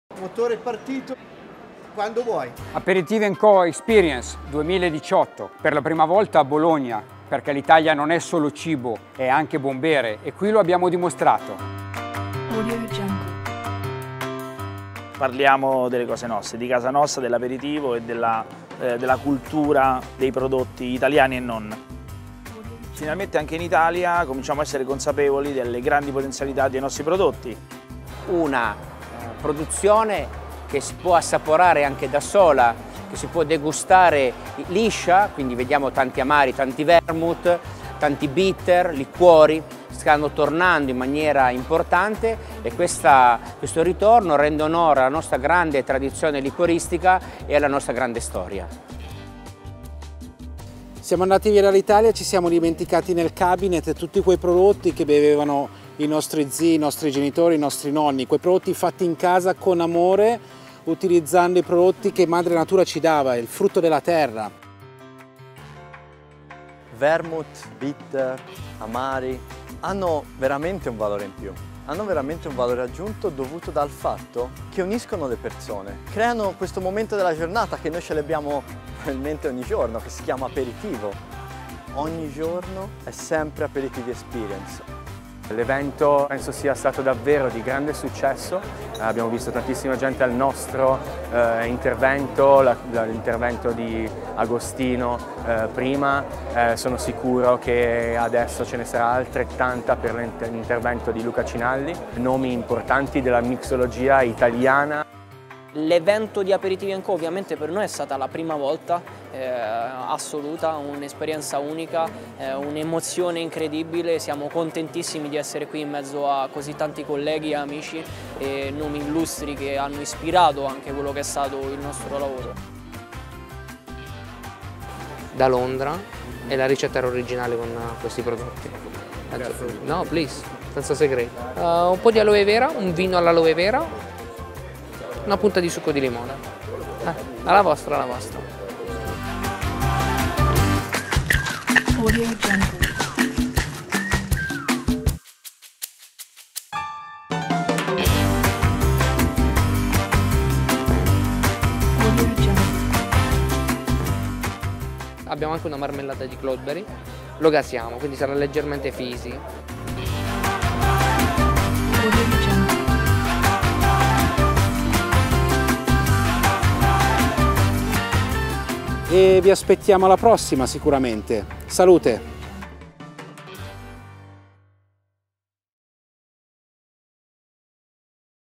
Aperitivi&Co Experience 2018 - Documentary
Il più importante evento trade dedicato al mondo di aperitivi, digestivi e corroboranti fa di nuovo centro: dai grandi vermouth all’immensa tradizione liquoristica italiana e internazionale, il viaggio attraverso il buon bere nostrano ha appassionato e coinvolto il pubblico di fico eataly world